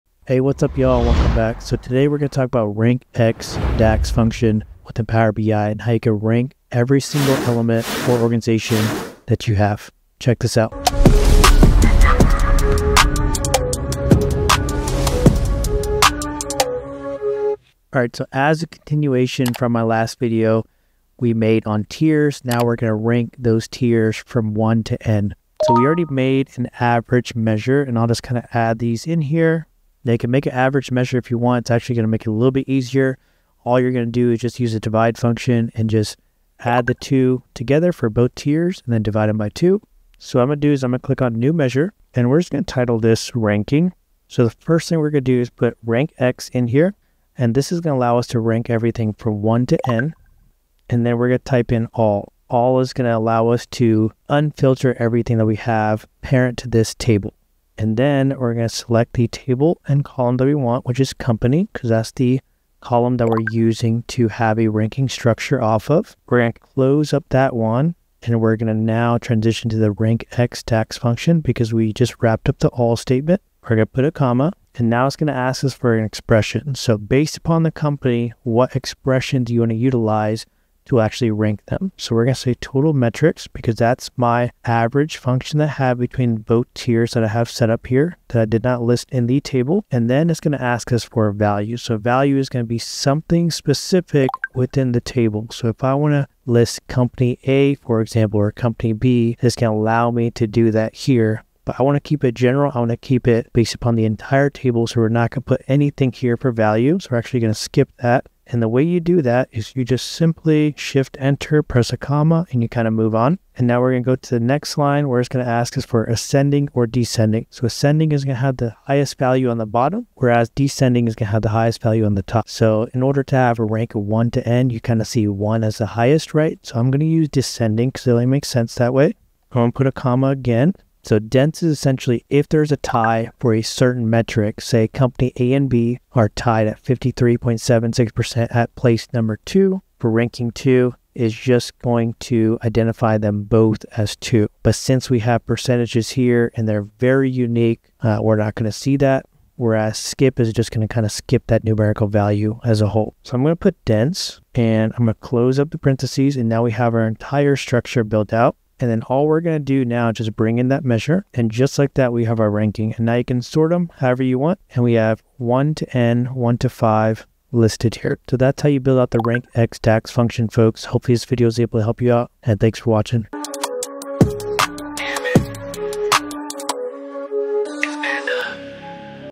How To Rank Anything In Power BI Super Fast!
Want to know who's performing best? This video breaks down the RANKX function in Power BI to help you rank teams, departments, or metrics in seconds. Real DAX, real examples, no fluff.

🧠 In This Video:
• What RANKX does
• The role of table, expression, order, and tie-handling
• When to use DENSE vs SKIP ranking
• Common mistakes and how to fix them
• A real example using [Total Metrics %] to rank companies

✅ Easily highlight top performers in your visuals.

💬 Drop a comment if you use performance tiers in your reporting.